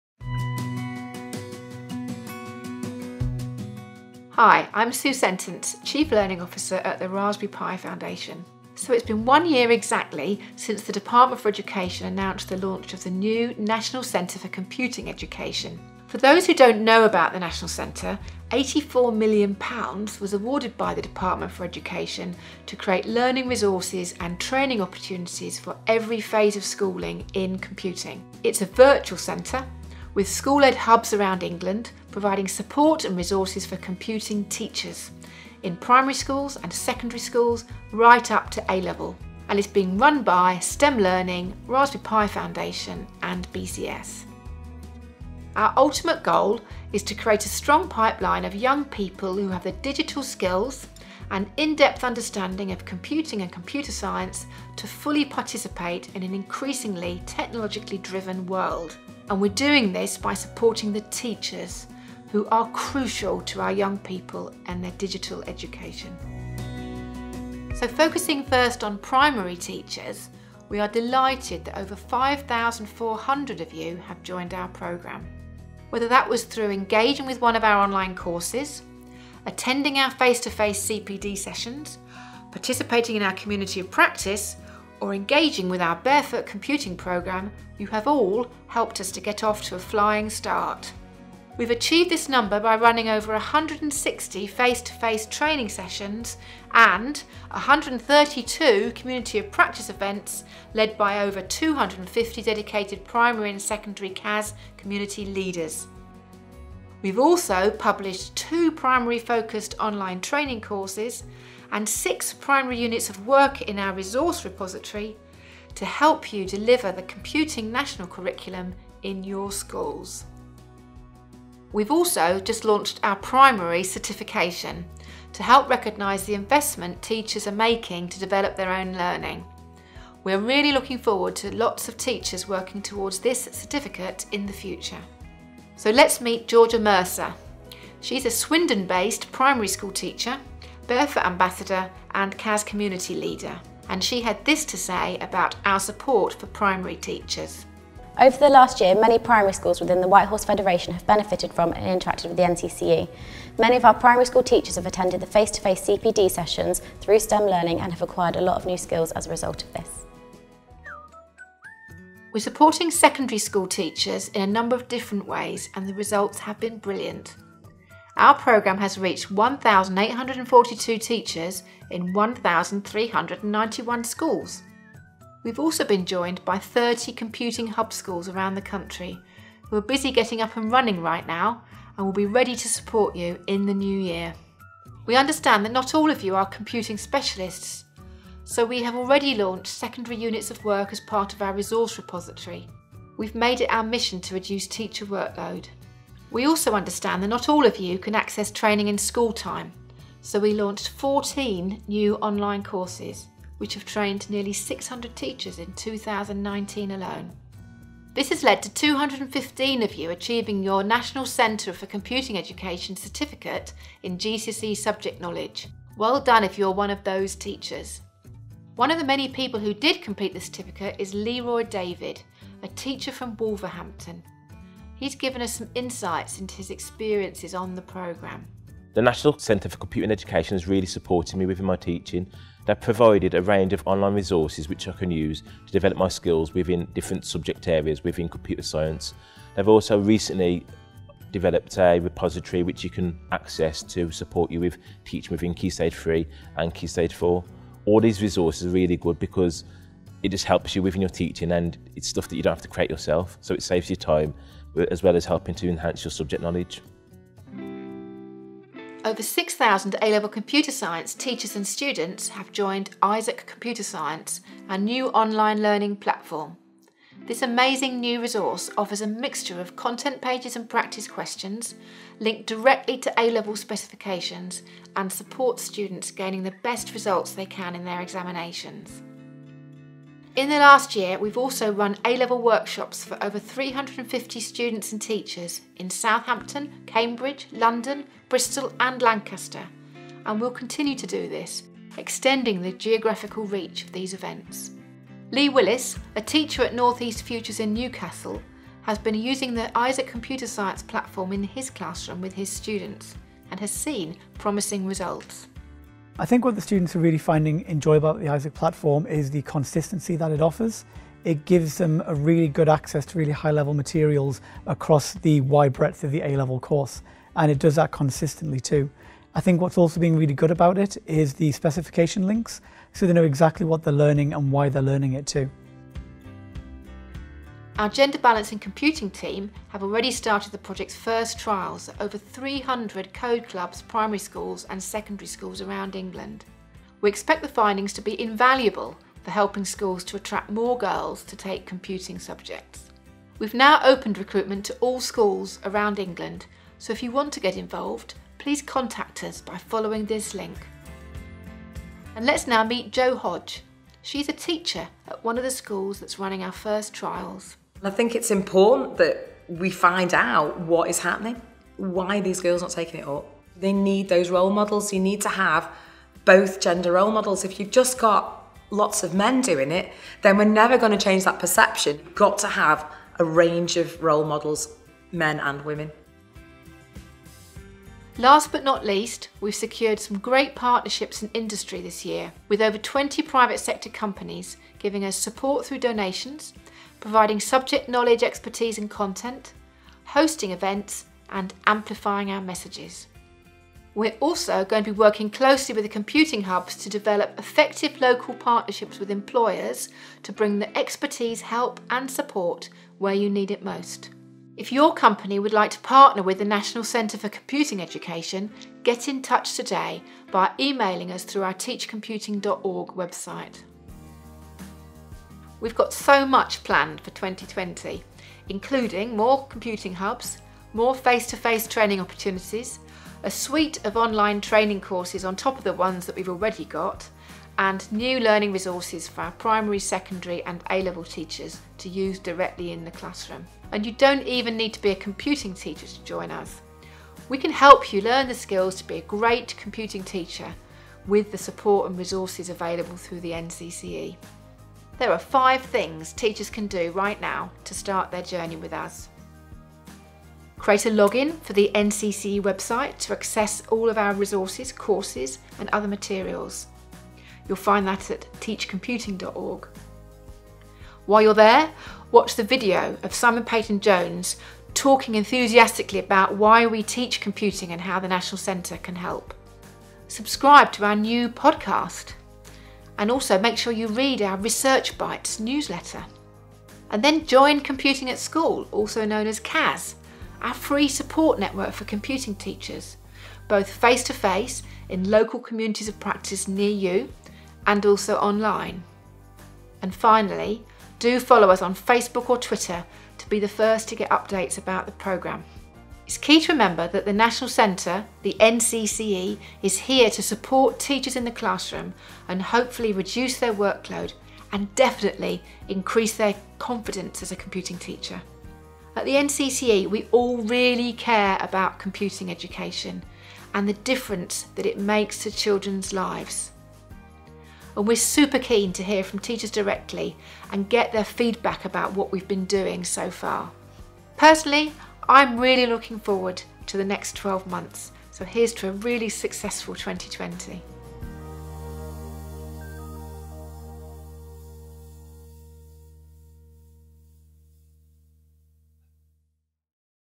National Centre for Computing Education: The Story So Far
We take a look back at the first incredible year at the National Centre for Computing Education. Discover how we are supporting teachers by giving them the tools and confidence to inspire the next generation of young learners.

► Want to learn more about Isaac Computer Science for A level students and teachers?